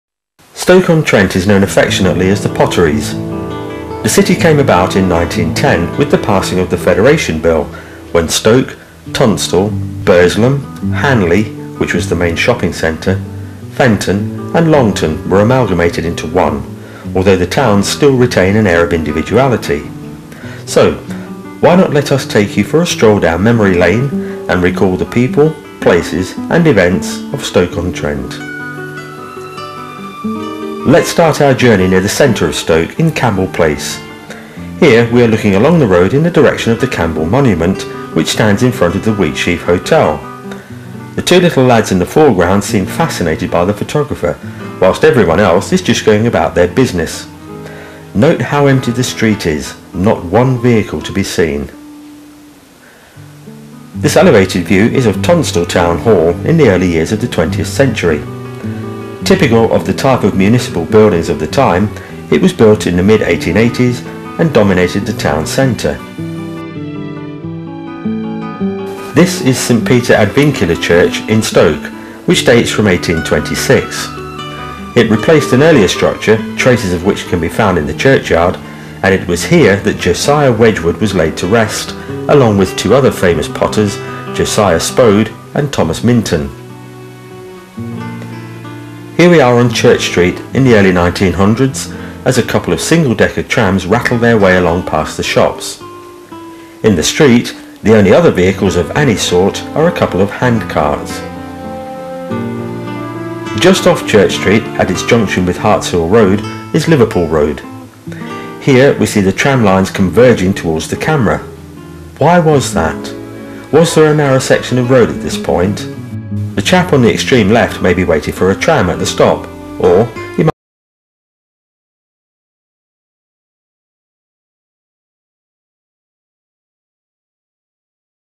stoke on trent-Past in pictures
Let Past in Pictures take you on a nostalgic journey into Stoke-on-Trent's magnificent past.